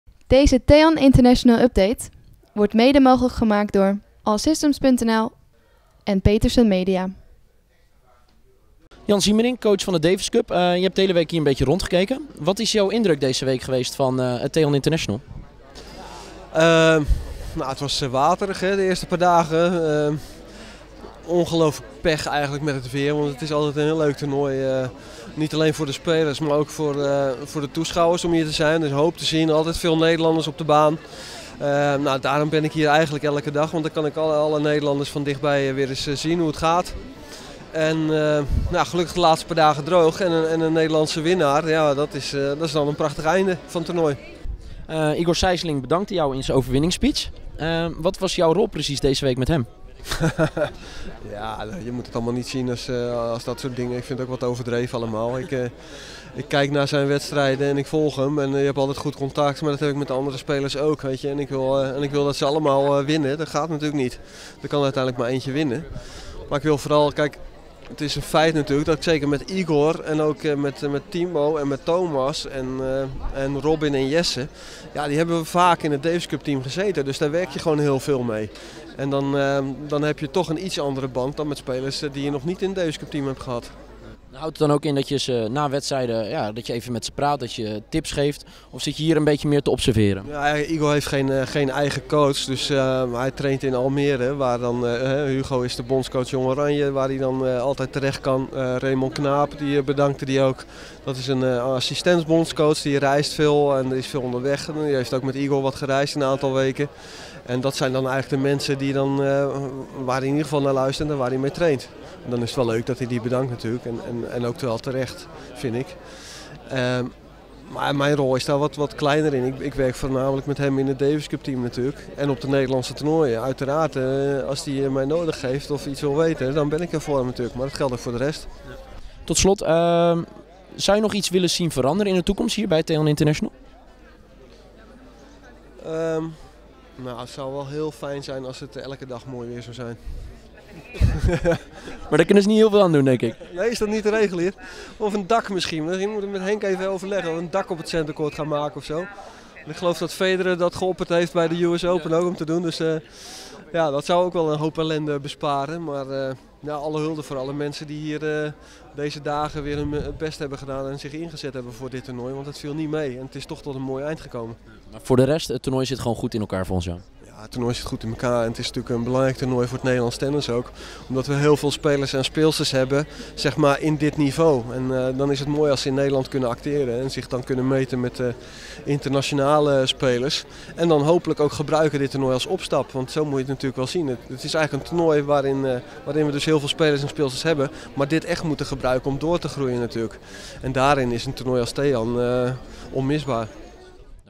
Siemerink: 'TEAN International is onmisbaar'
Jan Siemerink, coach van het Nederlandse Davis Cup team, was de afgelopen week elke dag aanwezig tijdens het TEAN International. Op de slotdag liet hij zijn mening schijnen over het Challenger toernooi: 'Het TEAN International is onmisbaar'.